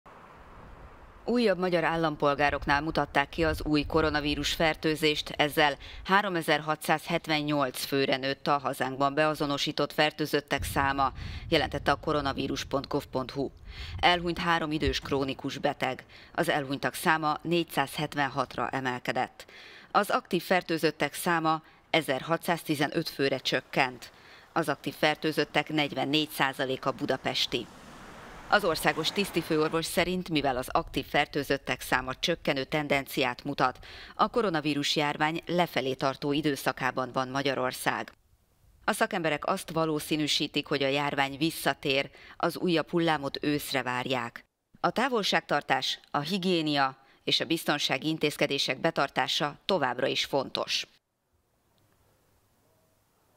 Koronavírus: csökken az aktív fertőzöttek száma Magyarországon
A szakemberek azt valószínűsítik, hogy a koronavírus-járvány visszatér, az újabb hullámot őszre várják.